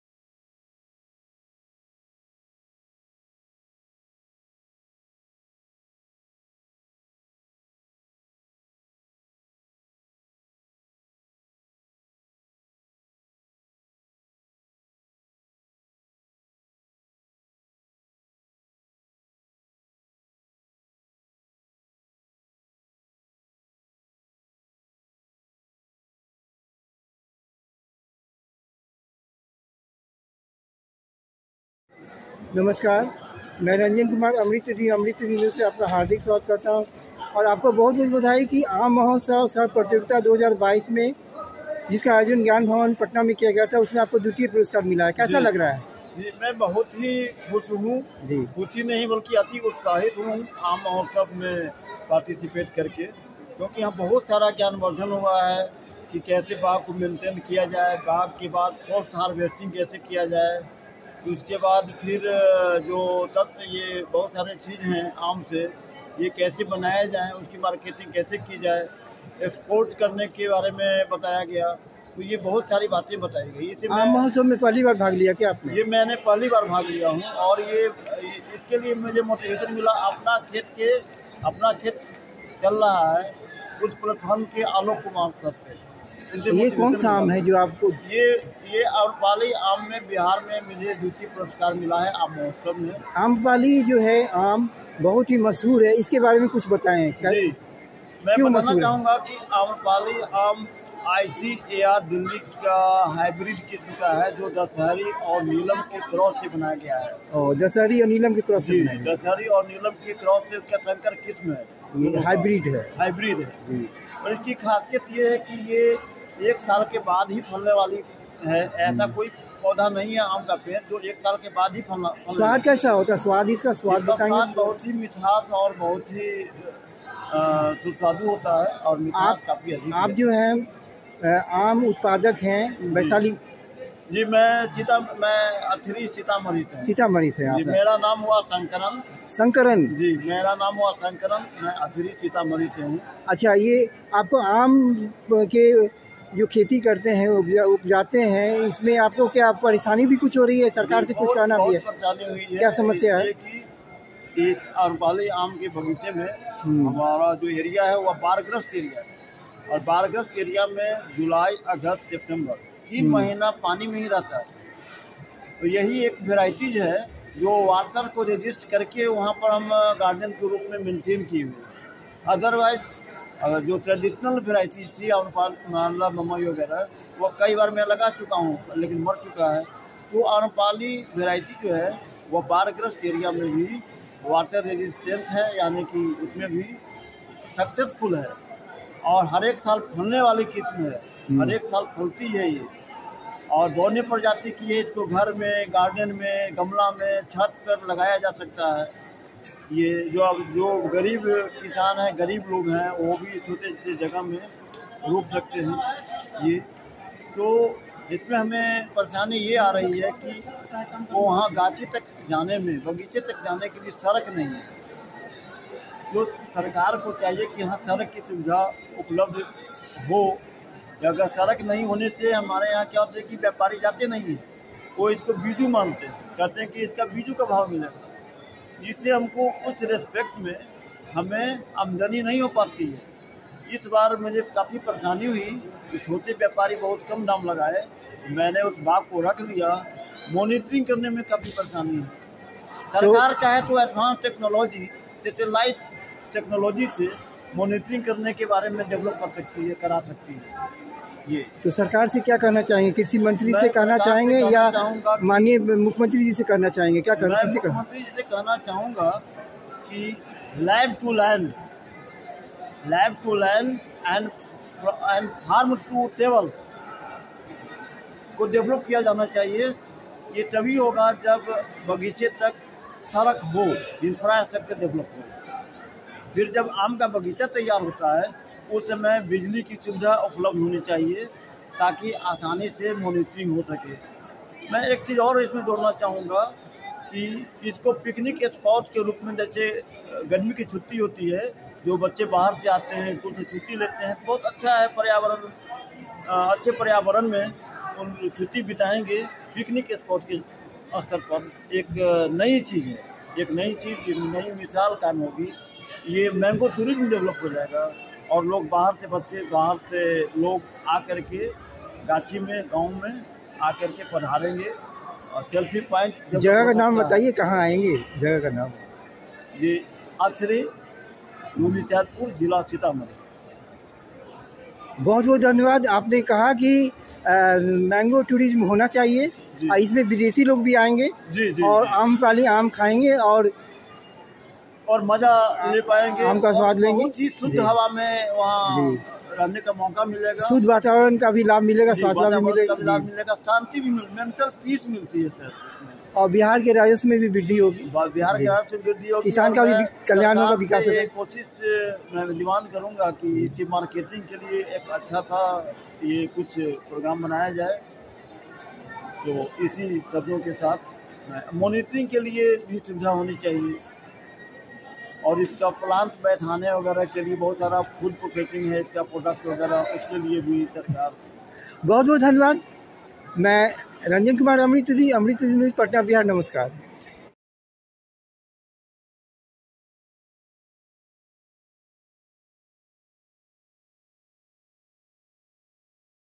🙏💐🥭 Better Road is essential for MANGO TOURISM in Athari, Sitamadhi |SHANKARAN, Mango farmer
This Hindi Video is based on an Interview with SHANKARAN Second Prize winner Mango Farmer in Amrapali Species Group in Aam Mahotsav Bihar-2022, Gyan Bhawan, Patna.

Interviewer | Ranjan Kumar Amritnidhi, MSc

Second Prize Winner in Amrapali Mango Species Group | SHANKARAN